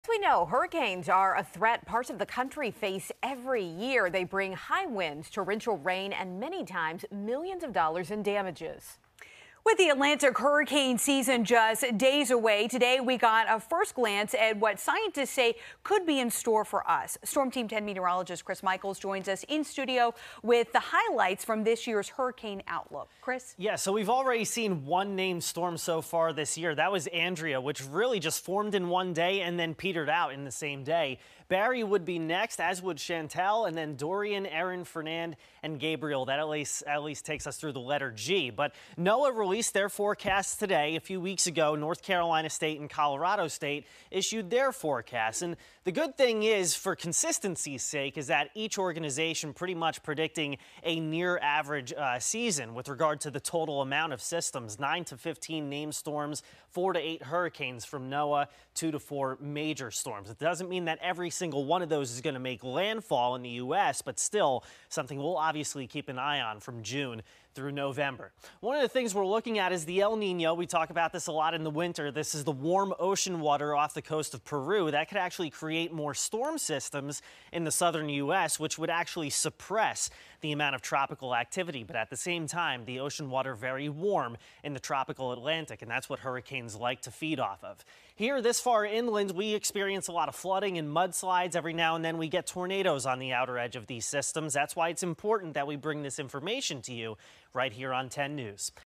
2019 NOAA Hurricane Outlook released
It's expected to be a near-average season.